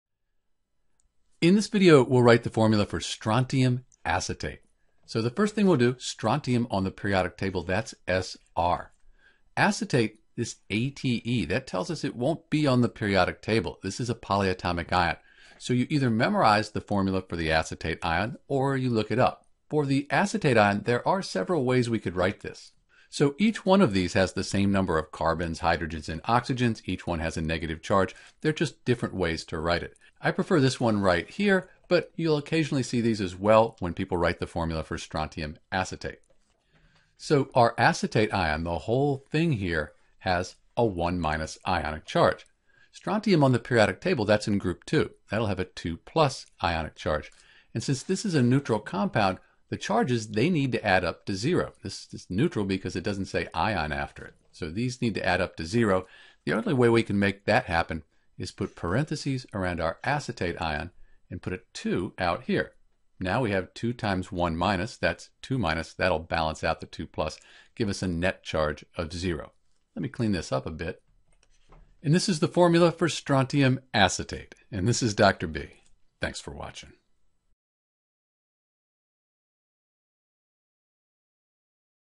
How to Write the Formula for Strontium acetate
In this video we'll write the correct formula for Strontium acetate (Sr(CH3COO)2). To write the formula for Strontium acetate we’ll use the Periodic Table and follow some simple rules.

Keys for Writing Formulas for Binary Ionic Compounds:

1. Write the symbols for each element.
2. Find the charge for each element using the Periodic Table. Write it above each element. For help finding charge, watch
3. See if the charges are balanced (if they are you're done!)
4. Add subscripts (if necessary) so the charge for the entire compound is zero.
5. Use the crisscross method to check your work.

Notes:
For a complete tutorial on naming and formula writing for compounds, like Strontium acetate and more, visit: